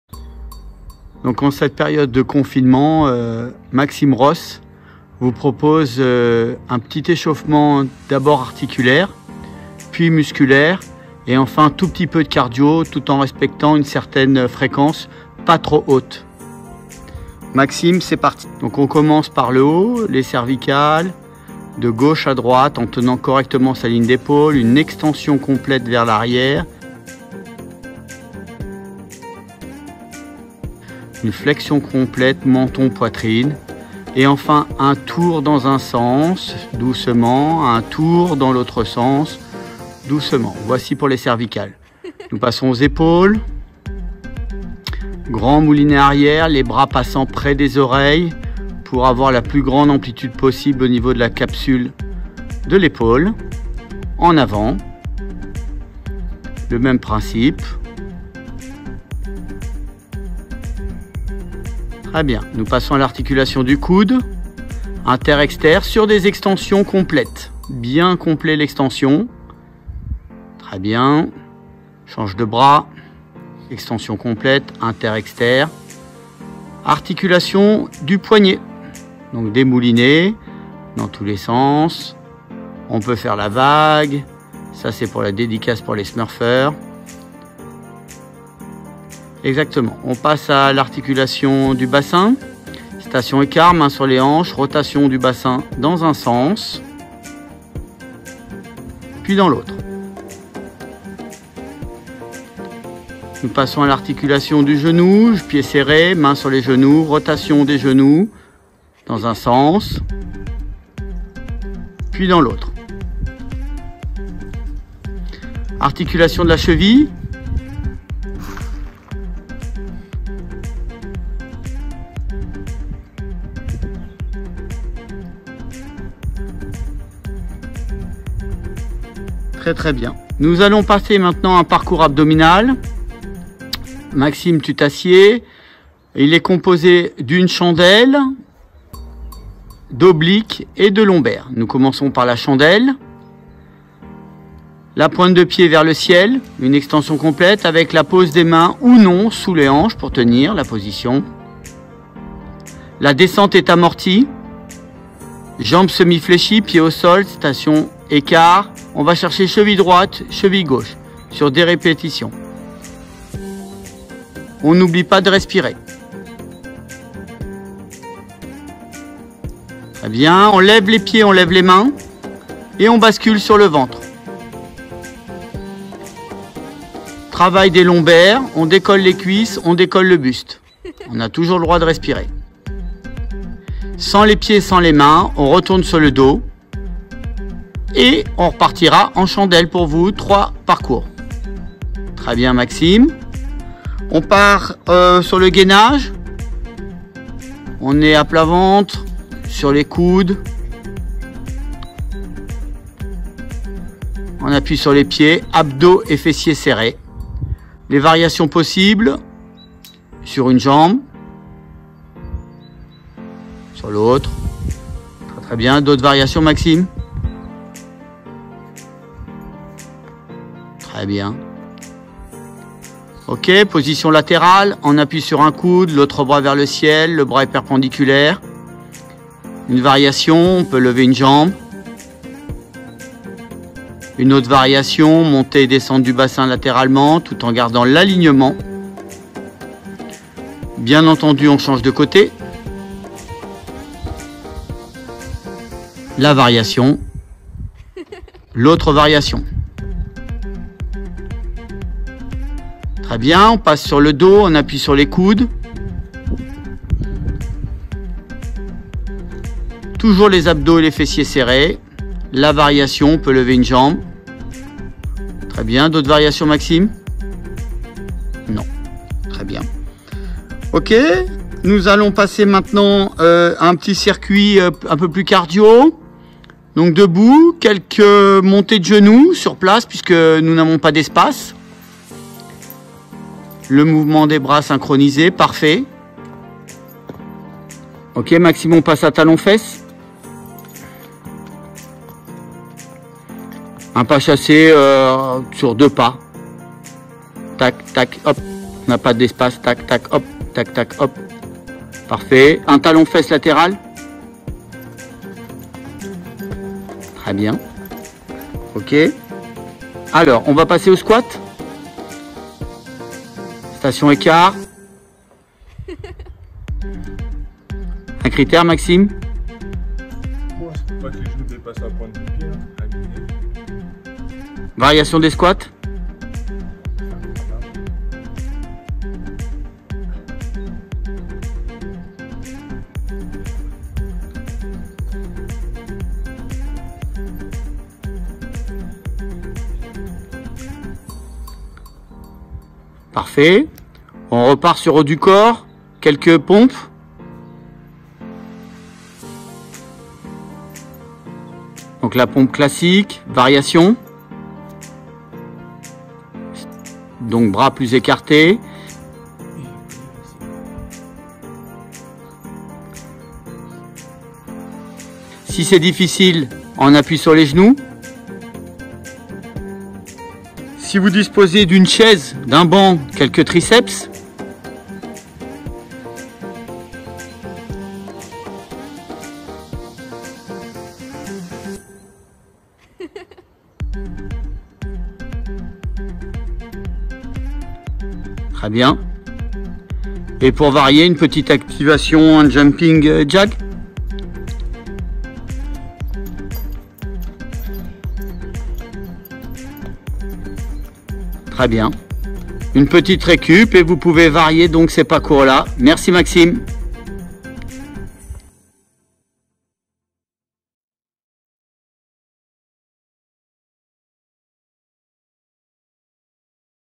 Tous en Forme avec le basketteur Maxime Roos
Maxime Roos est basketteur professionnel au Metropolitans 92 Boulogne Levallois en Jeep Élite et vous présente un enchaînement d’exercices :
- échauffement articulaire
- parcours musculaire
- exercices cardios
Voix Off, Thierry Barrière, professeur d’éducation physique et sportive et co-directeur du service des sports d’Université de Paris